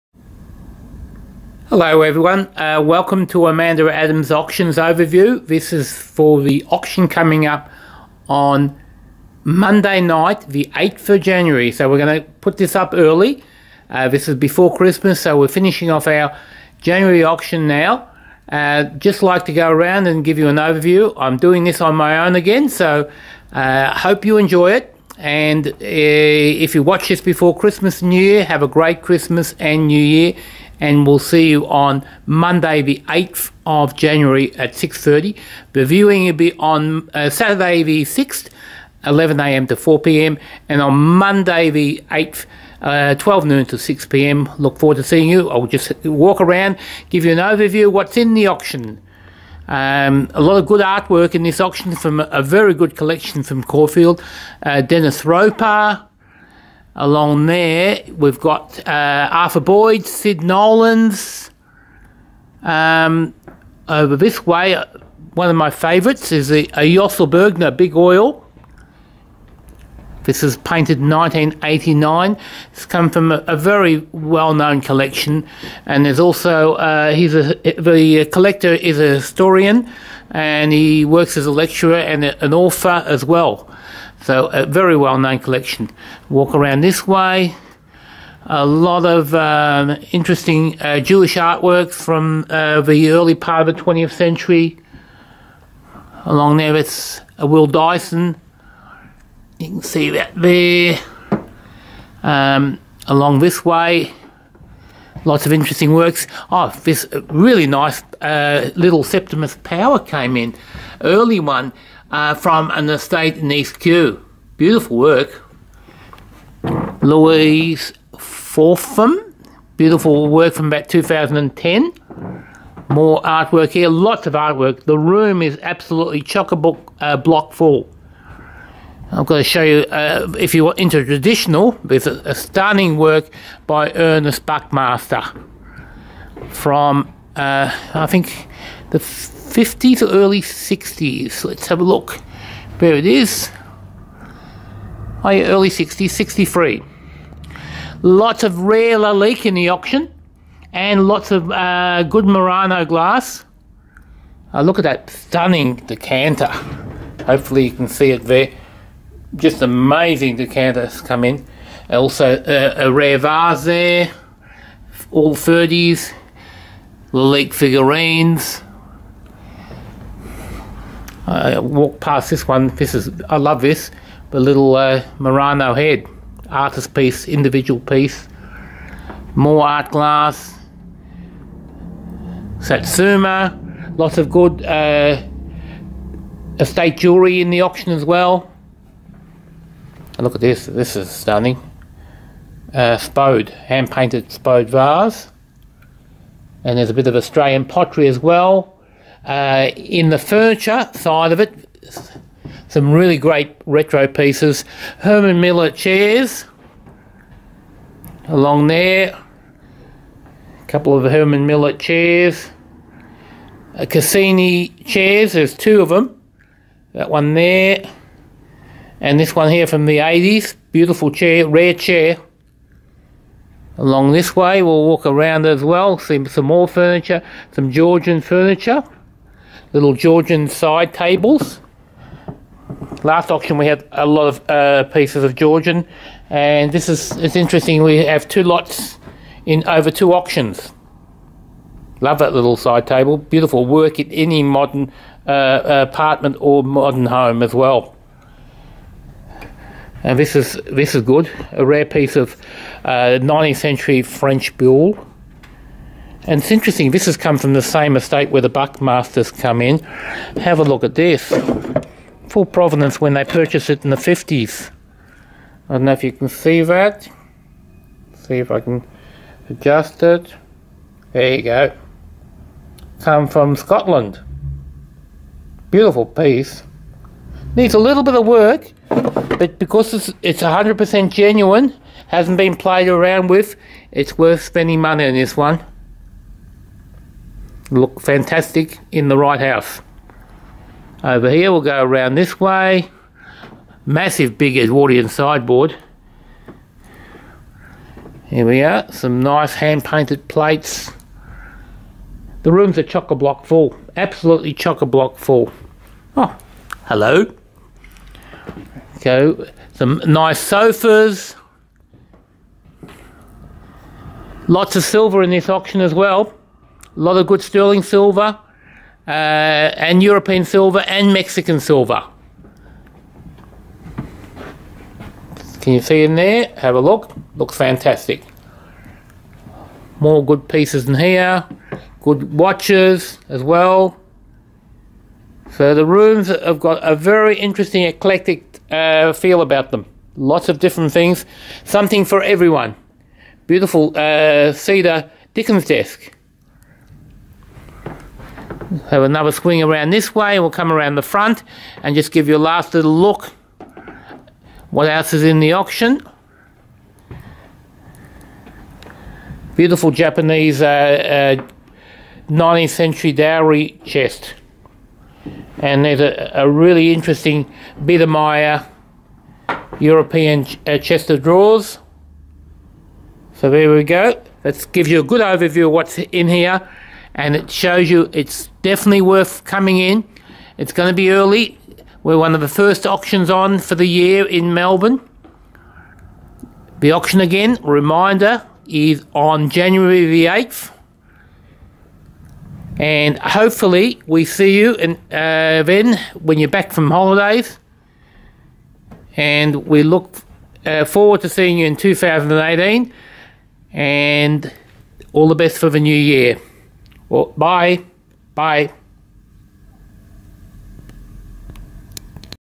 AMAZING START FOR 2018 AT AMANDA ADDAMS AUCTIONS
Amanda Addams Auctions – Monday 8th January 2018- Auction starts 6-30 pm

Viewing Saturday 6th 11am to 4 pm. Monday 8th -12 noon to 6 pm
Outstanding and important auction a great start to 2018
 Includes the estates: Estates: Kew East,

Contents of collector's collections: Kew, Caulfield, Prahran, Toorak, Balwyn.

Silver:  Continental, English, Asian, cutlery, caskets, candlesticks, bowls, candlesticks etc.

Decorative wares:  large collection of Rare Lalique fantastic Murano, Royal Doulton, Royal Worcester, Moorcroft, Crown Derby,

Jewellery: Huge selection of estate jewellery includes: precious stones and diamonds, costume, stone, silver and sterling silver, pearl, shell, cameos, rings, bracelets, earrings, necklaces and pendants, Danish, English, Italian etc.

Art works: Louise Forthum, David Bromley, Ernest Buckmaster, Septimus Power, James Swinton Diston, Arthur Boyd, Sid Nolan, Yosl Bergner, Adrian Kerfoot, Dennis Ropar, Will Dyson, Santos, Steinhardt, Gutman, Chagall, etc.

Oriental wares:  Cloisonné, Satsuma, ceramic vases, figures, Japanese Studio pottery, Kutani, jewellery, stone carvings, etc.

Furniture: Designer chairs by Herman Miller, Cassina, Russell Casper, Retro Italian and Australian designer furniture , Georgian Sideboards, Antique French Boulle Bureau with superb Provenance, Swedish wedding chest, and antique fruitwood Japanese Wedding chest   Antique and Vintage Australian and European  , quality, tables, chairs, armchairs, chests, side tables, sideboards, sofas, etc.

Ceramics and pottery: Includes Australian vintage and retro vases, bowls etc,

Other:  Bronze figures, clocks and watches, mirrors, Persian rugs, costume jewellery, timber carvings, tapestries, firescreens, light fittings, etc.

You must come –An outstanding auction, not to be missed.
Next auction will be on the 5th of February 2018
344 High Street Kew Victoria 3101